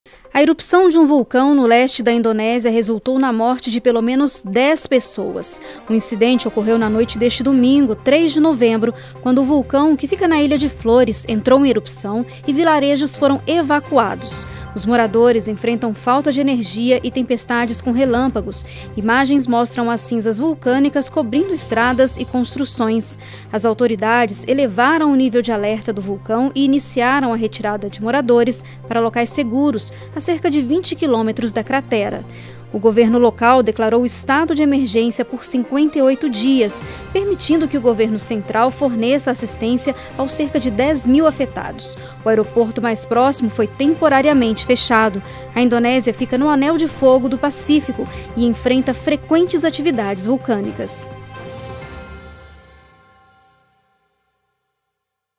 Erupção do vulcão Lewotobi Laki-laki na Indonésia deixa 10 mortos
A erupção de um vulcão no leste da Indonésia resultou na morte de pelo menos 10 pessoas. O incidente ocorreu na noite deste domingo, 3 de novembro, quando o vulcão, que fica na Ilha de Flores, entrou em erupção, e vilarejos foram evacuados. Os moradores enfrentam falta de energia e tempestades com relâmpagos. Imagens mostram as cinzas vulcânicas cobrindo estradas e construções.

As autoridades elevaram o nível de alerta do vulcão e iniciaram a retirada de moradores para locais seguros, a cerca de 20 km da cratera. O governo local declarou estado de emergência por 58 dias, permitindo que o governo central forneça assistência aos cerca de 10.000 afetados. O aeroporto mais próximo foi temporariamente fechado. A Indonésia fica no “Anel de Fogo do Pacífico” e enfrenta frequentes atividades vulcânicas.